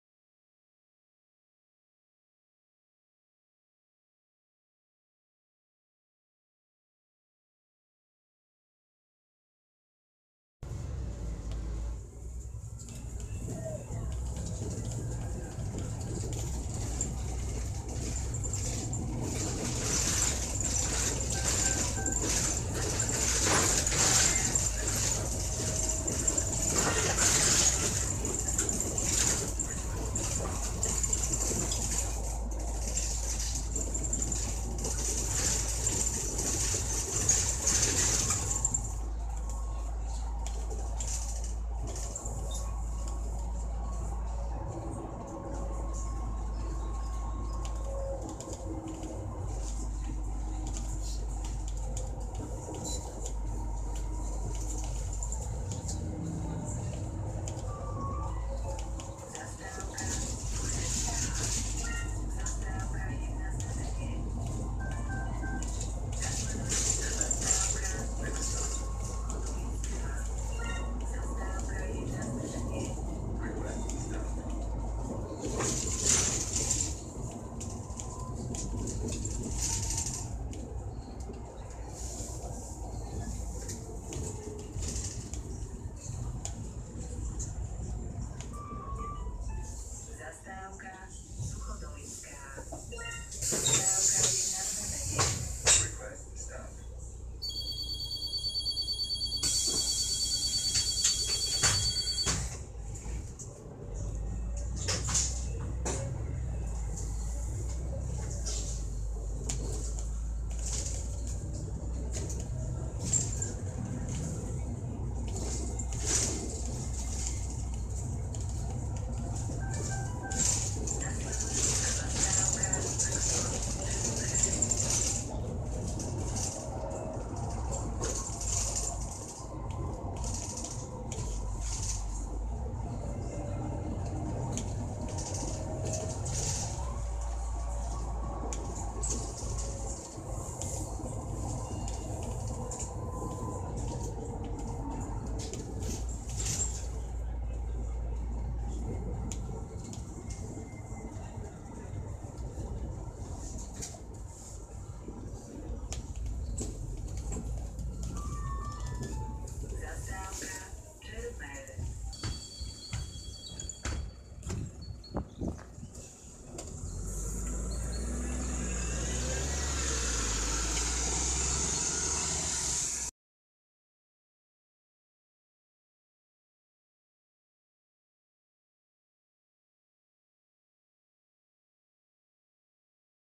Linka 14 : Havlíčkova - Čermeľ .
Dnes tu mám pre Vás linku 14 na trase : Havlíčkova - Čermeľ.
Zastávky :
🚏 Havlíčkova
🚏 Letecká ( na znamenie )
🚏 Suchodolinská ( na znamenie )
🚏 Čermeľ

Typ autobusu : Sor NB 12 city
Evidenčné číslo : #5716

Natočené : 6.4.2019
V čase : odchod z Havlíčkovej : 10:29

Keď sa Vám video páčilo , tak nezabudnite dať like 👍 a komentár .

MATO PATO 2019 .